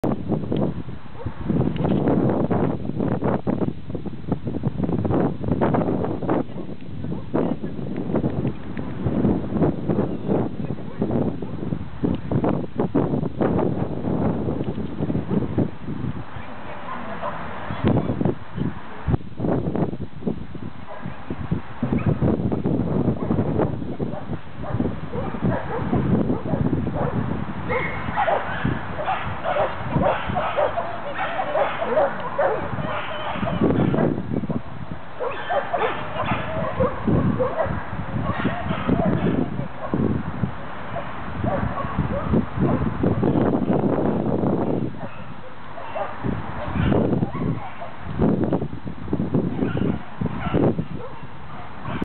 Chaconne and me
A little play with the four months old Chaconne. The beginning was very hard, because he always wanted to herd all the dogs who were moving.